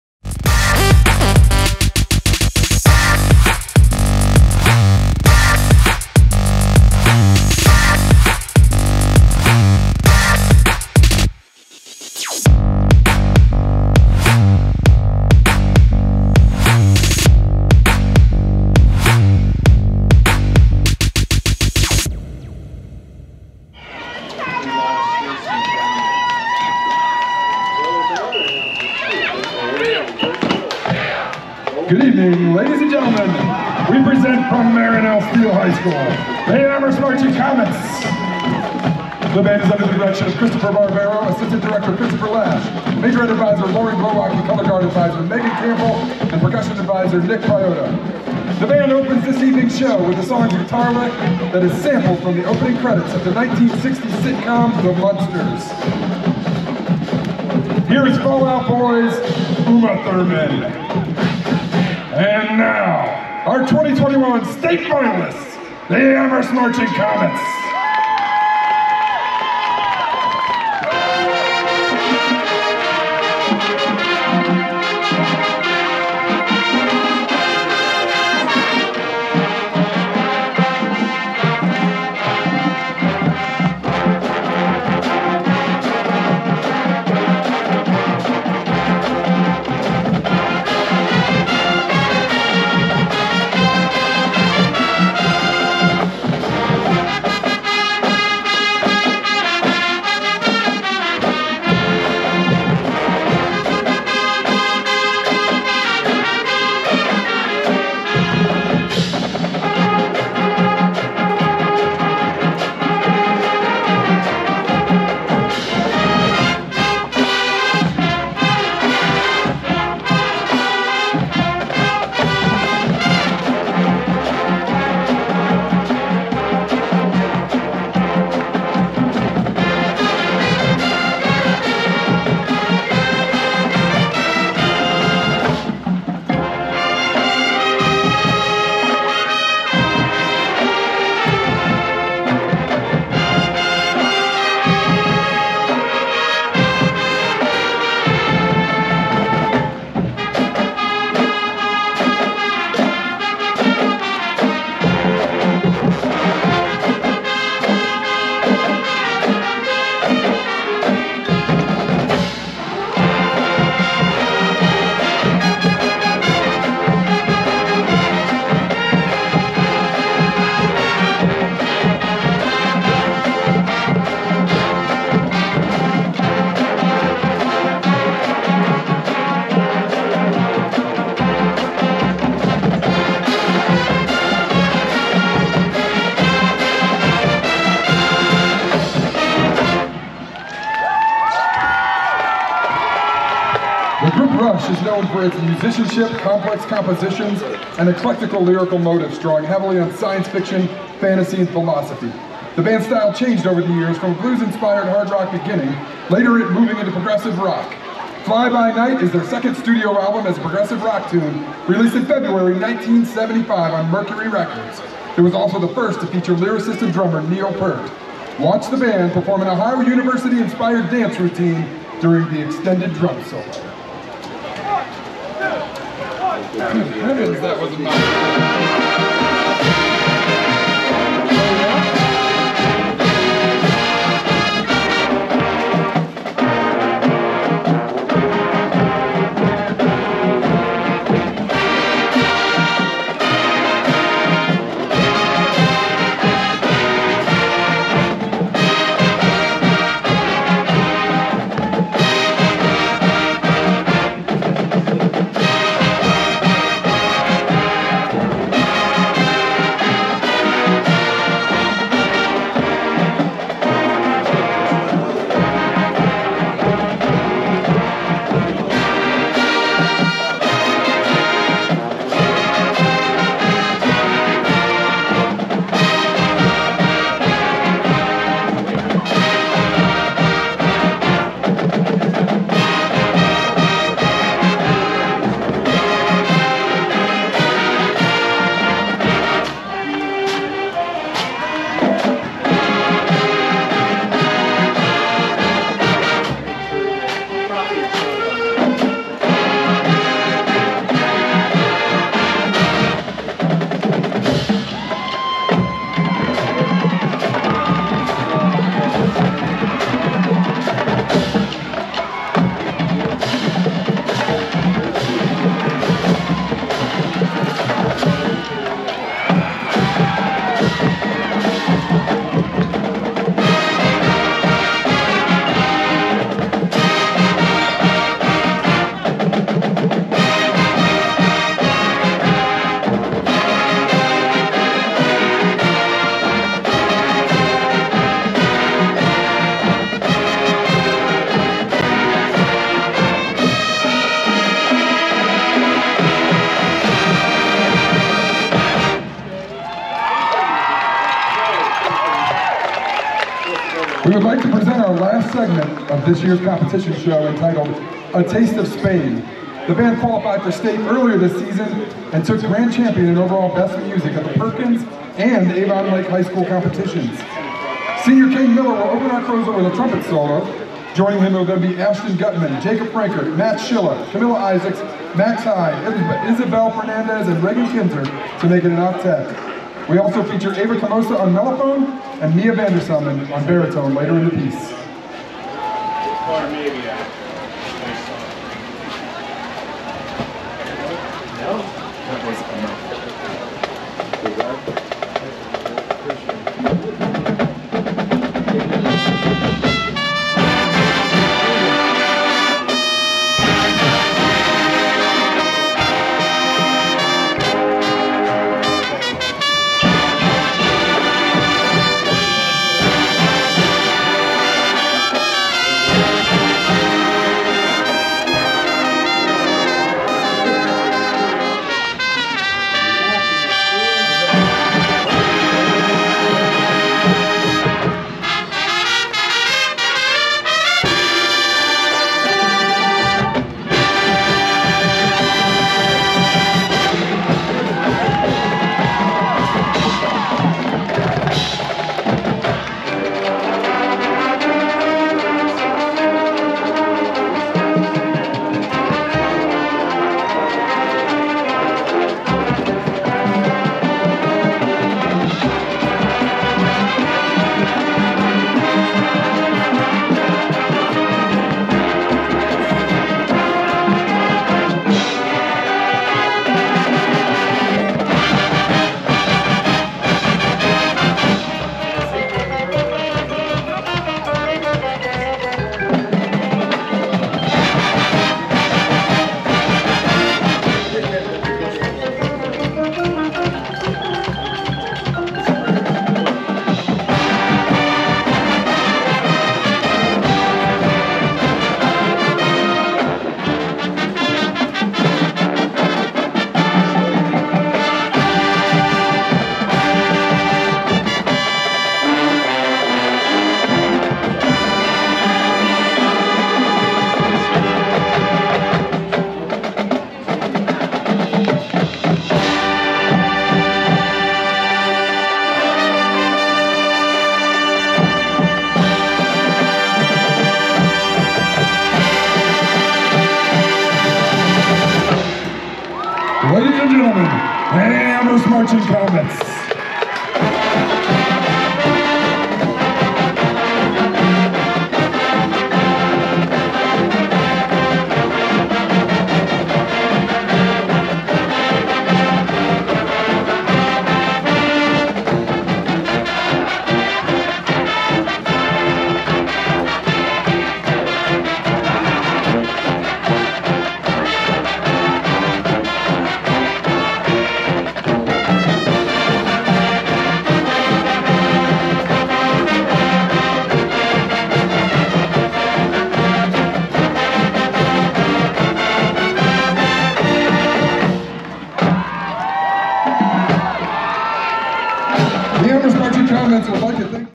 Game 9 - October 15 - Away at Elyria
It's game 9, and there is a short delay for lightning, but that doesn't stop the Marching Comets from delivering a SHOCKING performance!